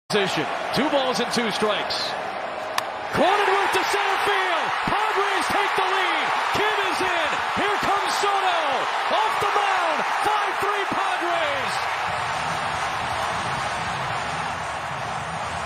Jake Cronenworth RBI SINGLE GIVES THE PADRES THE LEAD | LA Dodgers @ SD Padres GAME 4
NL WILDCARD GAME 4 HIGHLIGHTS